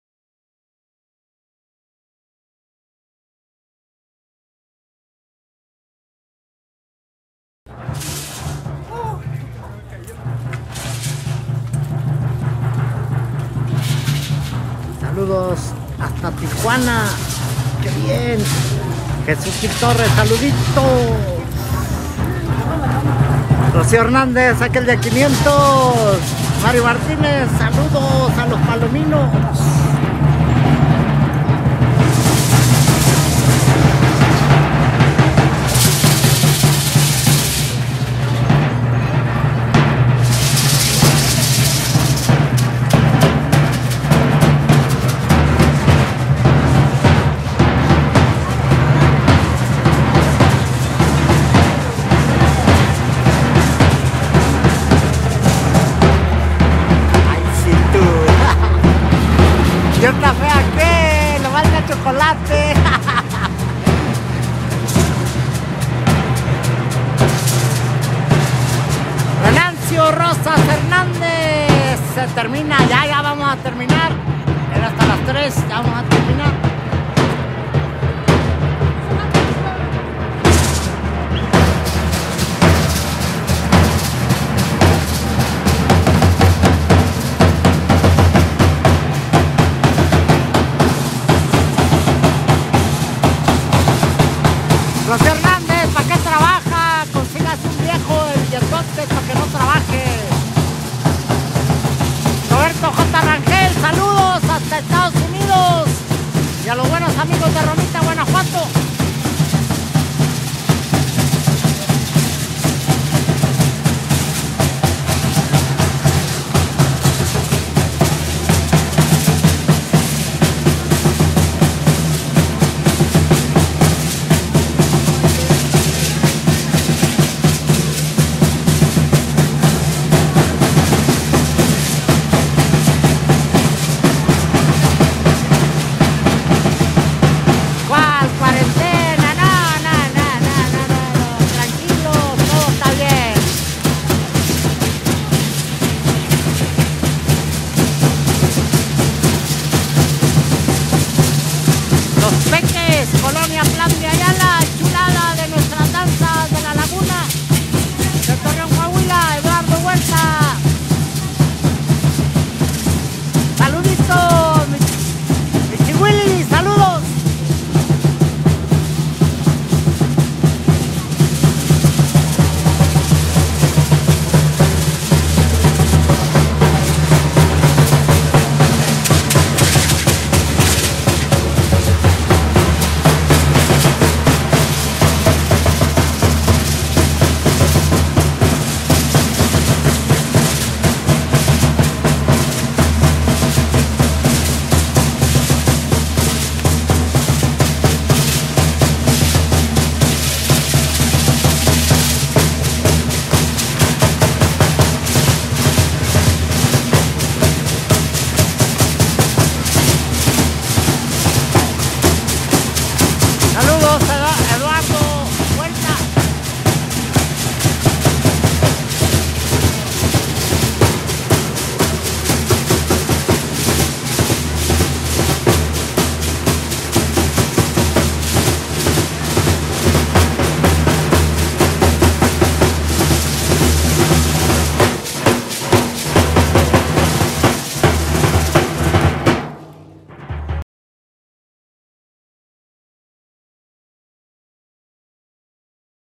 En La 1o. de Mayo "LOS PEKES" de Torreón, Coah.
Matlachines “Los Pekes” de La Col. Plan de Ayala, Torreón, Coah. Fiesta del Sr. San José. Col. 1º. De Mayo, Torreón, Coah. 22 de marzo 2020.